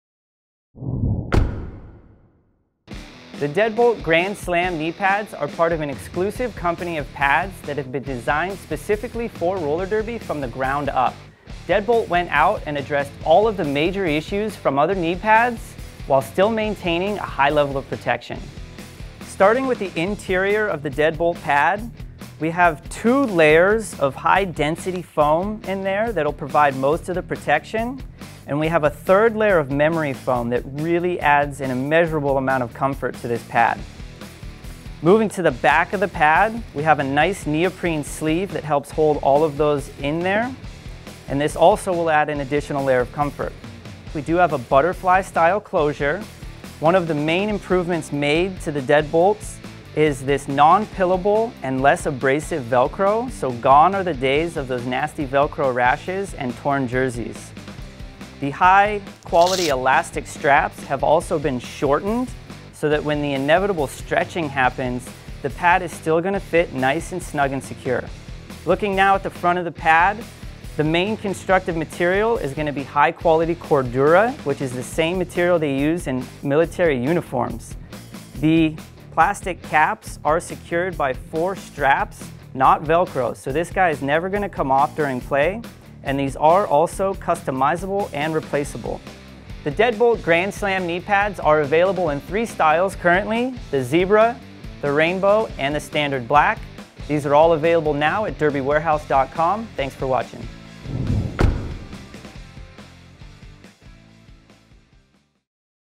Deadbolt Grandslam Knee Pads Review
Deadbolt Grand Slam Knee Pads are designed for derby from the ground up. By listening to the common complaints about derby knee pads, Deadbolt was able to create a design that addressed those issues and provides incredible protection.

Stand out features include: Removable cover for washing, customizable clear caps, shorter elastic straps to anticipate stretching and super soft memory foam interior padding that acts like a built in knee gasket!